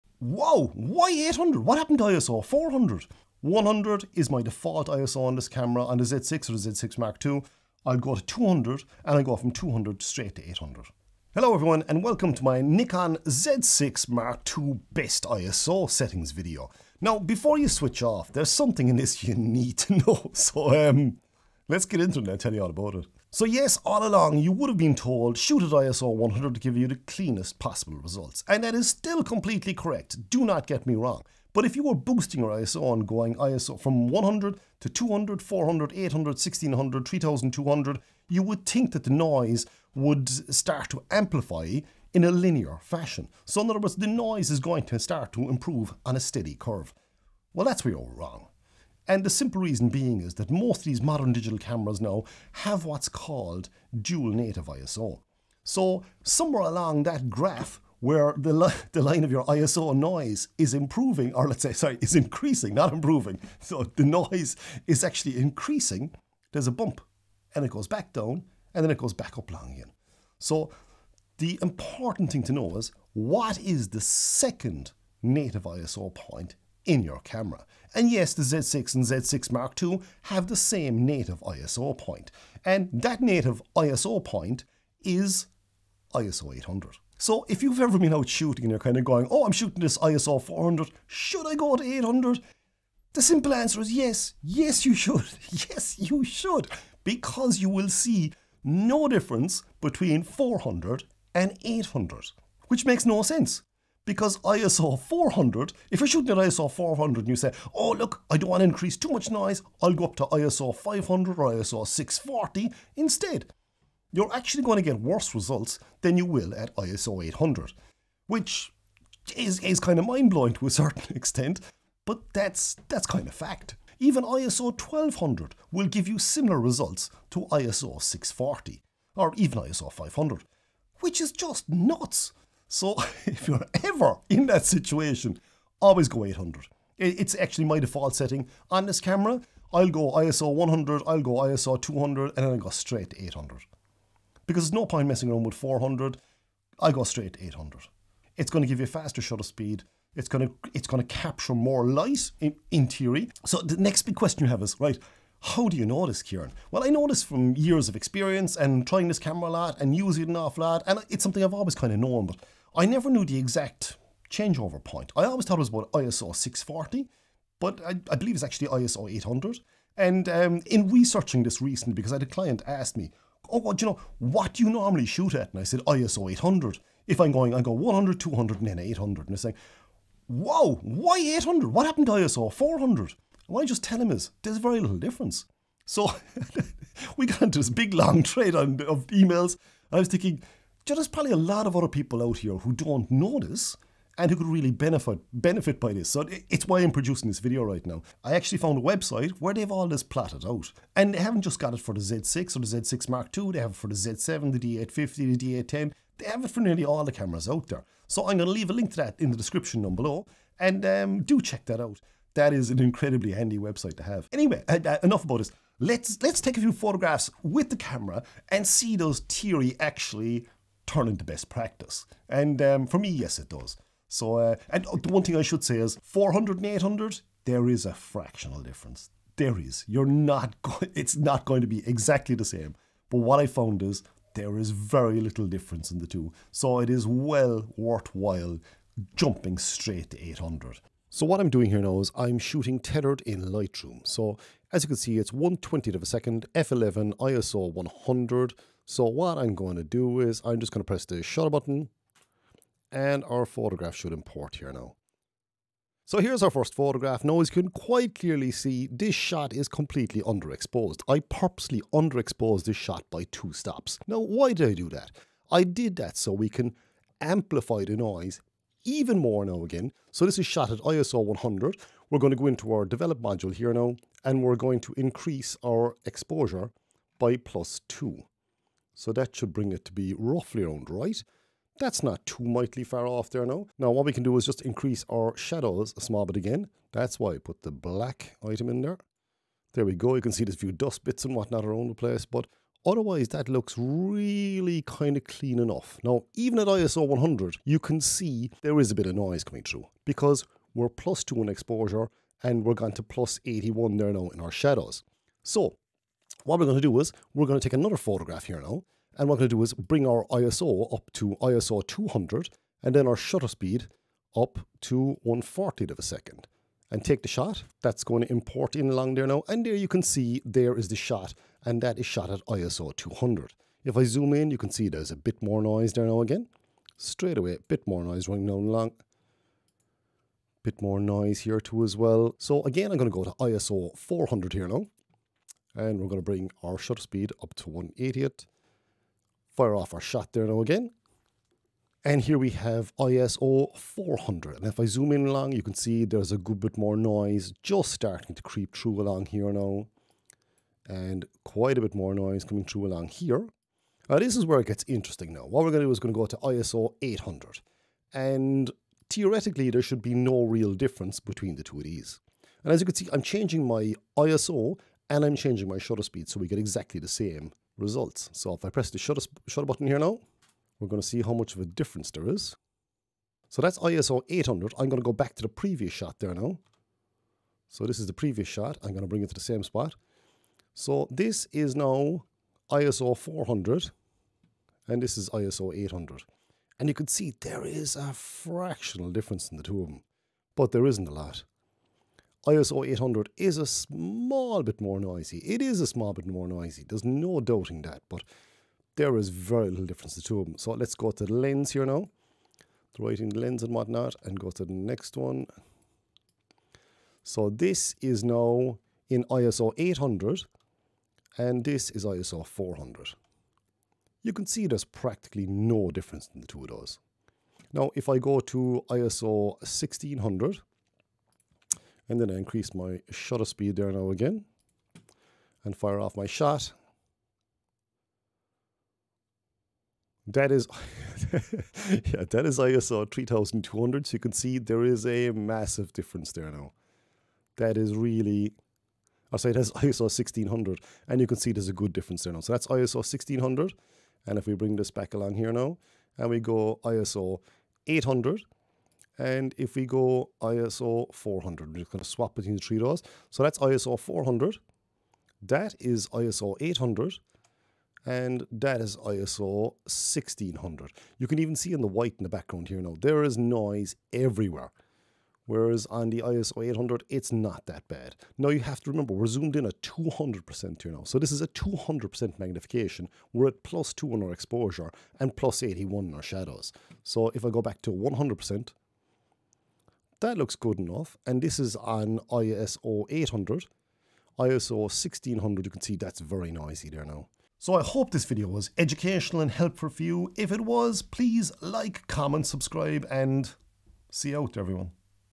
Nikon Z6 & Z6 ii ISO Tutorial, The best ISO setting! Did you know this?
My Nikon Z6 and Z6 ii ISO tutorial and best settings video where I talk you through a little trick I have for getting the best ISO performance from your camera.
It seems that not a lot of people are aware of the dual native ISO feature in the Nikon Z6 and Z6 ii cameras. They both share the same sensor so the ISO sweet spots are the same then.

Similar to the Nikon Z7 and the Nikon Z7 ii cameras although their ISO sweet spot is different as it is a different sensor with a far higher pixel density so the ISO performance as a result is slightly weaker.

All you have to do is to select your camera model on the right hand side and you will get all the details you need on screen in a very helpful graph.

0:00 Intro
0:23 ISO and why this is different.
1:18 Second Native ISO setting for Z6 and Z6ii.
3:19 Website Source.
4:00 Capturing the photographs.
4:30 ISO 100 Photograph
5:17 ISO 200 Photograph
5:42 ISO 400 Photograph
6:12 ISO 800 Photograph
6:20 ISO 400 VS ISO 800
6:48 ISO 400 vs ISO 800 vs ISO 1600
8:07 The Wink.